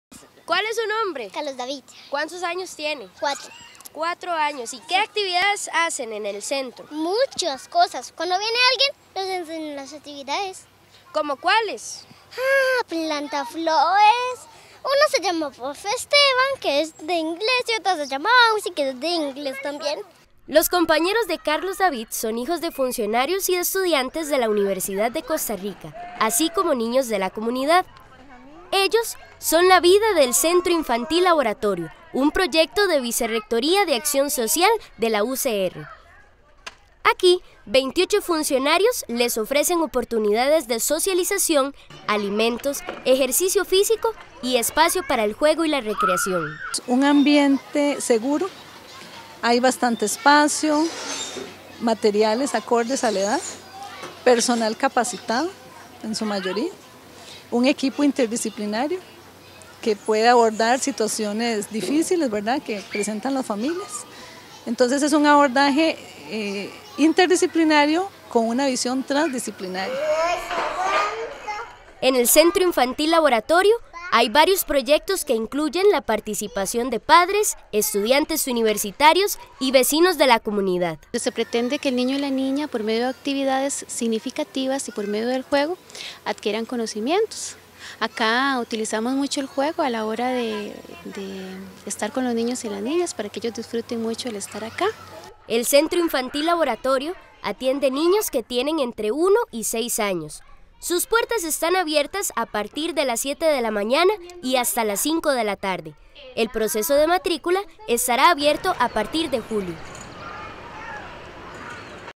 Centro Infantil Laboratorio UCR cumple 30 años 30/05/2013
Quienes asistieron por primera vez al Centro Infantil Laboratorio ya traen a sus hijos e hijas porque han pasado 30 años  desde que esta institución abrió sus puertas.
Aquí  niños y niñas de entre uno y seis años reciben cuido y atención que fomentan su desarrollo integral. [Periodista Kryssia León Solís]